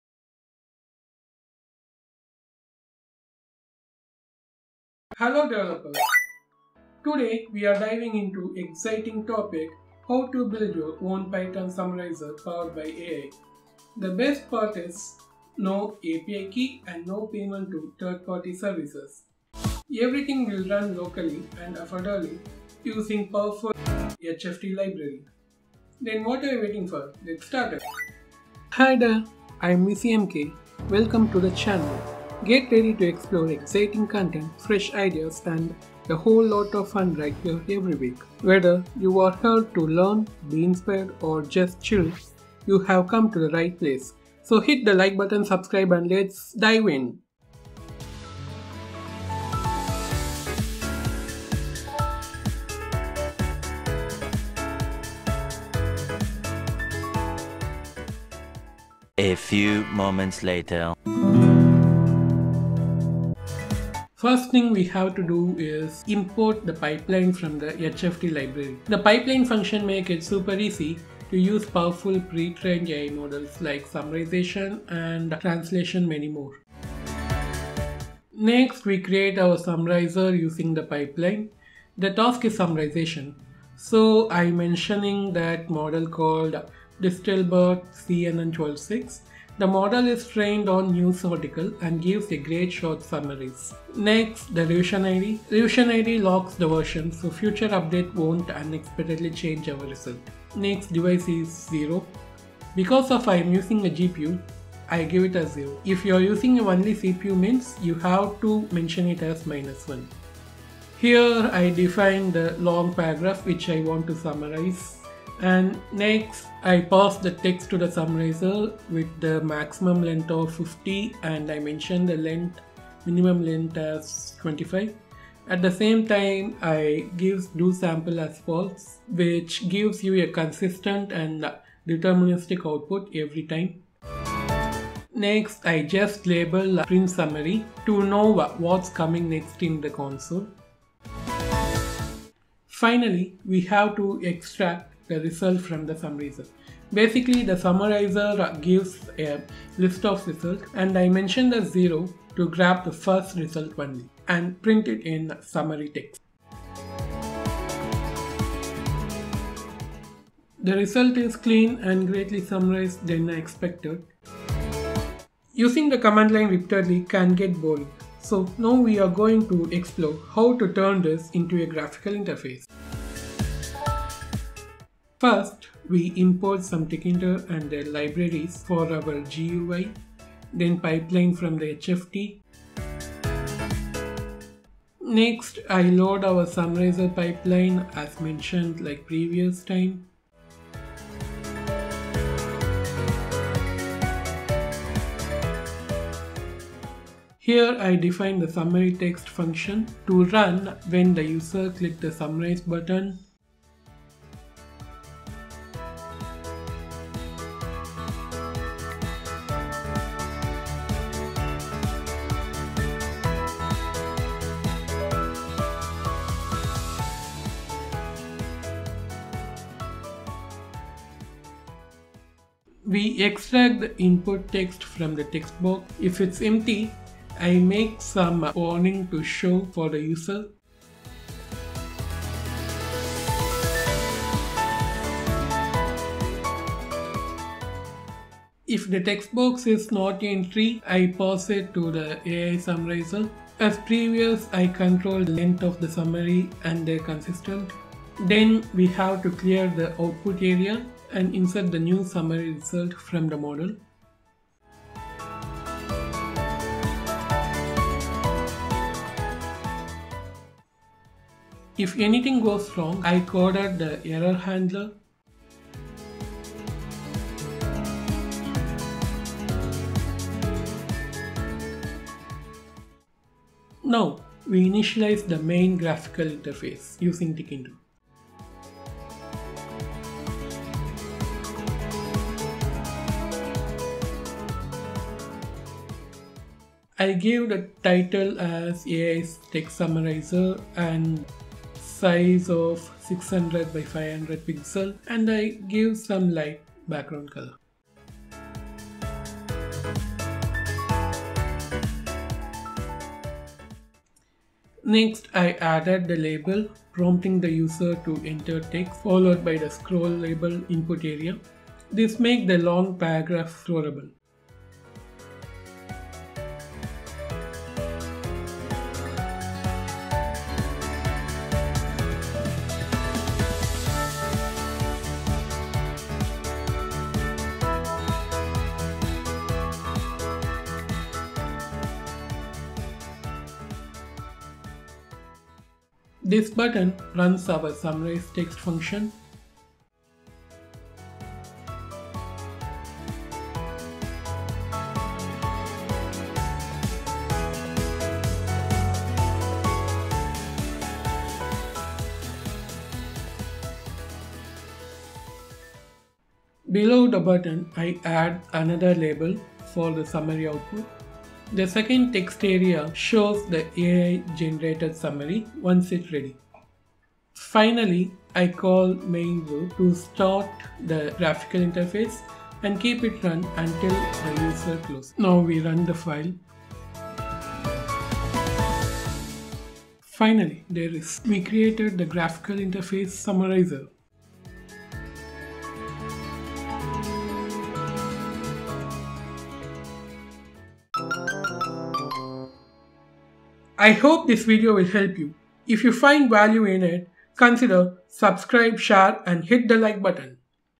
Build an AI Text Summarizer with Python – No API Required! | VCMK
Build an AI Text Summarizer with Python | Hugging Face Transformers + Tkinter GUI
Learn how to create your own AI-powered text summarizer using Python! This tutorial walks you through integrating Hugging Face Transformers into a clean desktop GUI using Tkinter. No API key required — the model runs completely offline!

💻 Setup Instructions

To follow along, use the following terminal commands:

python -m venv .venv
.\.venv\Scripts\activate        # For Windows users
pip install torch
pip install transformers
pip install pillow
python summarizer_gui.py

✅ Make sure to activate the virtual environment before running the script.

📁 Source code is available on my Instagram @techbyvcmk

✔️ Build a GUI text summarizer in Python using Tkinter
✔️ Use Hugging Face pipeline() for offline summarization
✔️ Summarize any paragraph instantly with just one click
✔️ Supports offline use (no API key required)
✔️ Customize the layout and output in a desktop-friendly format

🎞️ Credits & Info
🕒 Meme sound used at 0:57 is from: MyInstants
🎬 Except for the meme clip, all other content in this video is created and owned by TechByVCMK
📚 Purpose: Educational content only

🔧 Libraries Used

    transformers
    torch
    pillow (PIL)
    tkinter
    Python 3.9+